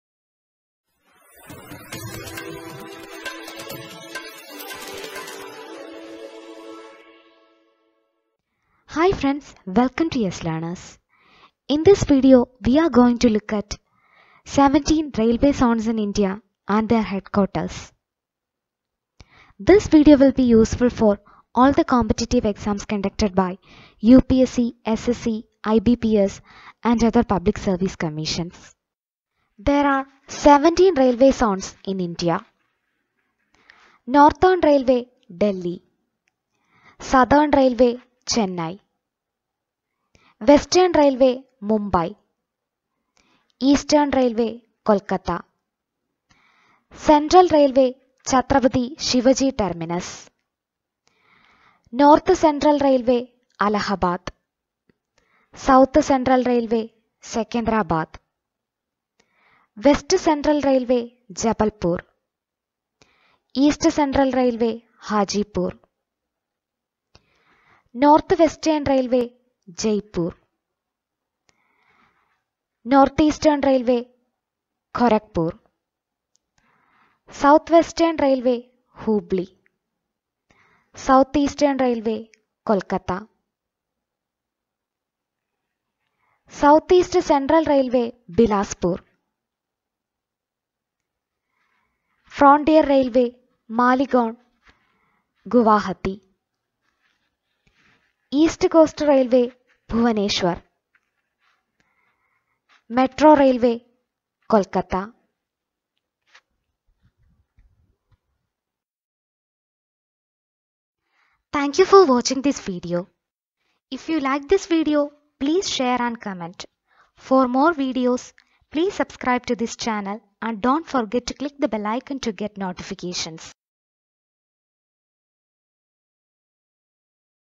Indian Railway Zones - Basic Things You Should Know
17 Railway Zones in India & their Head Quarters
Northern Railway –Delhi
North Eastern Railway – Gorakhpur
Northeast Frontier Railway – Maligaon (Guwahati)
Eastern Railway- Kolkata
South Eastern Railway – Kolkata
South Central Railway – Secunderabad
Southern Railway – Chennai
Central Railway – Mumbai
Western Railway – Mumbai
South Western Railway – Hubli
North Western Railway – Jaipur
West Central Railway – Jabalpur
North Central Railway – Allahabad
South East Central Railway – Bilaspur
East Coast Railway – Bhubaneswar
East Central Railway – Hajipur
Kolkata Metro – Kolkata

video on Indian railway zones and headquarters. Indian railway zones is a repeated section for SSC, PSC and Bank exams as well.

for news and information on latest SSC, PSC, UPSC, IBPS, Bank exams etc.